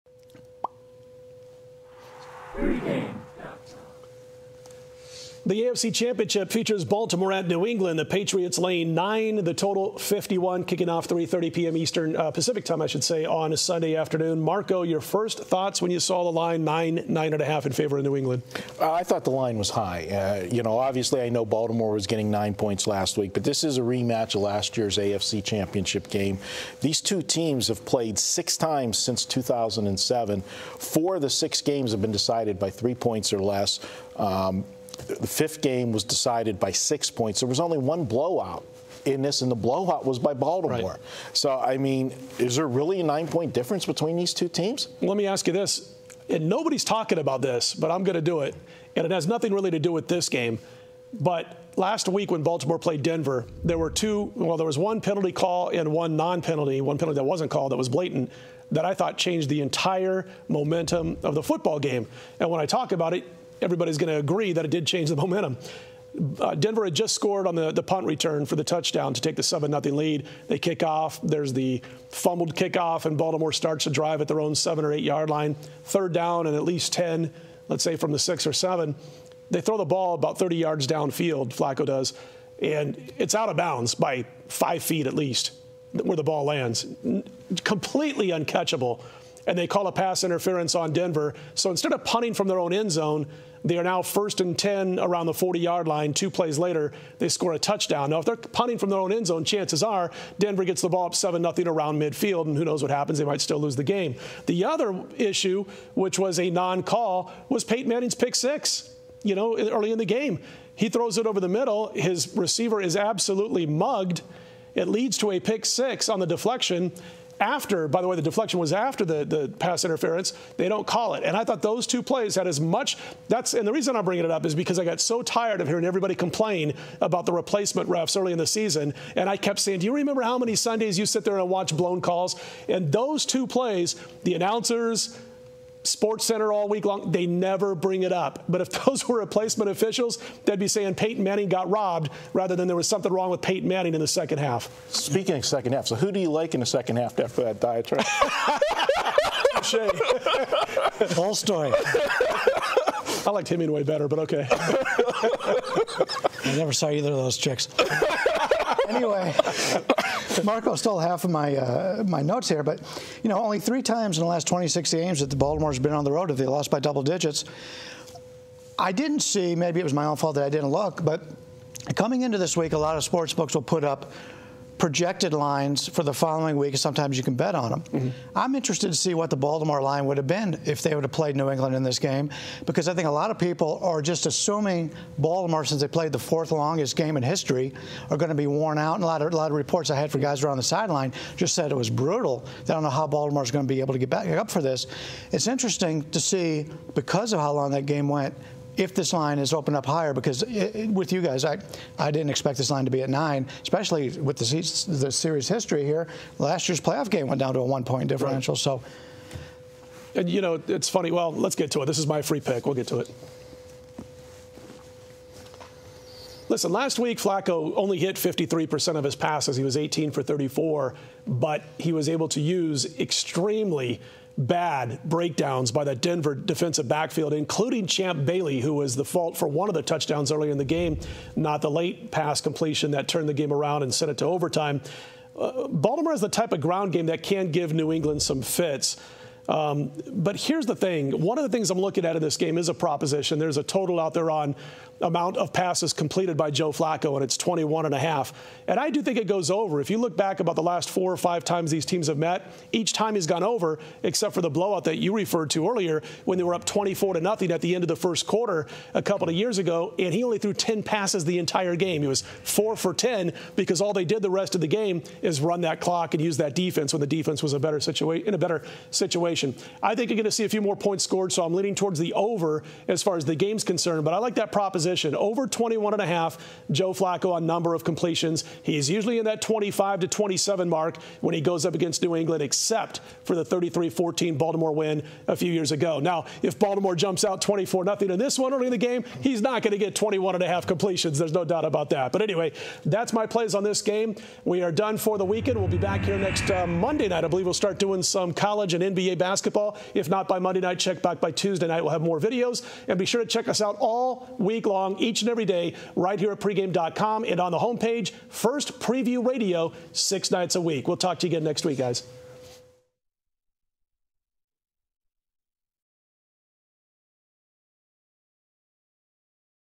NFL Free Picks: Baltimore at New England Betting
What pro football bettors need to know about Baltimore Ravens at New England Patriots on Sunday (1/20/13) - a free pick from Pregame.com's Scott Spreitzer, with Marco D'Angelo and Bryan Leonard.